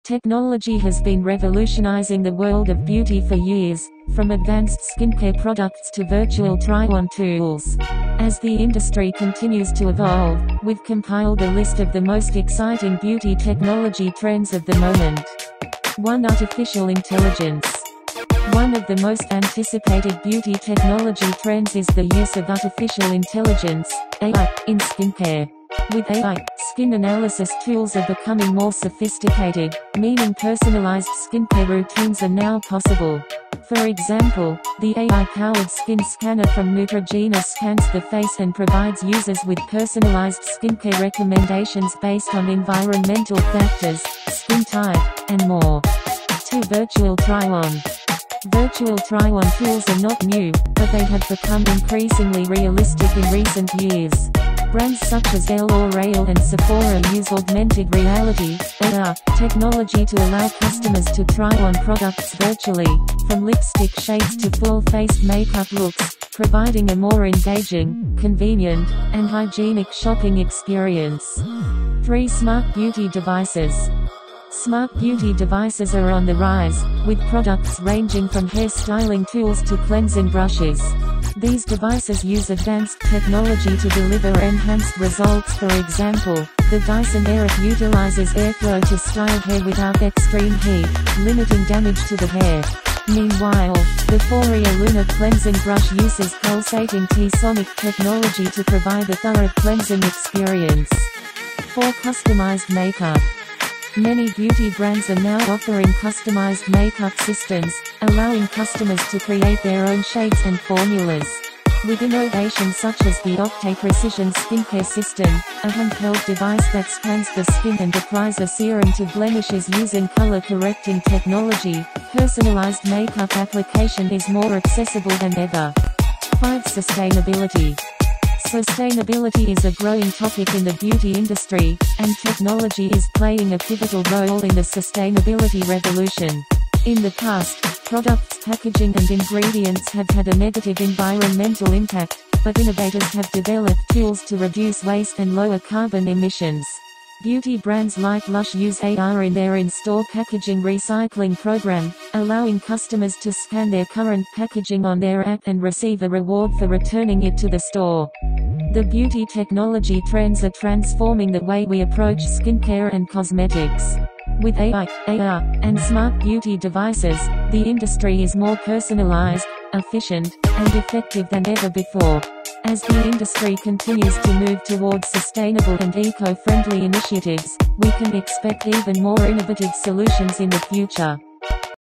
Top 5 Beauty Technology Trends You Need To Know About This Year | w1o9 VIRAL TRENDING - ojermes
The beauty industry has been revolutionized by emerging beauty technologies. In this video, we bring you the top five beauty technology trends that are making waves this year. From high-tech skincare devices to virtual makeup try-ons, we'll show you everything you need to know about the future of beauty. Discover the most innovative beauty tools and techniques that can transform your skincare routine and elevate your overall appearance. Unlock your inner beauty potential with the help of technology!